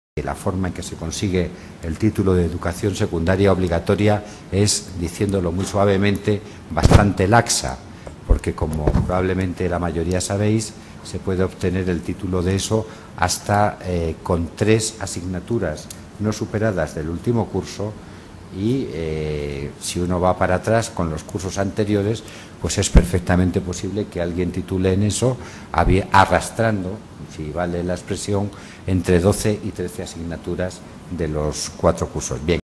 wert titulacion en la ESO. La ignorancia convertida en argumento
En el Campus FAES de 2012, El Ministro Wert habló de la Reforma educativa que tiene en mente. Un trozo de su intervención es este. En él demuestra que no tiene ni la más remota idea de cómo se obtiene el título de Graduado en Educación Secundaria Obligatoria. Lo malo es que toda su reforma la basa en su ignorancia.
Por si le interesa, Wert se puede leer artículo 15.2 del REAL DECRETO 1631/2006, de 29 de diciembre, por el que se establecen las enseñanzas mínimas correspondientes a la Educación Secundaria Obligatoria.
15. 2. Quienes superen todas las materias de la etapa obtendrán el título de Graduado en Educación Secundaria Obligatoria. Asimismo podrán obtener dicho título aquellos que hayan finalizado el curso con evaluación negativa en una o dos materias[EN TODA LA ESO], y excepcionalmente en tres, siempre que el equipo docente considere que la naturaleza y el peso de las mismas en el conjunto de la etapa no les ha impedido alcanzar las competencias básicas y los objetivos de la etapa.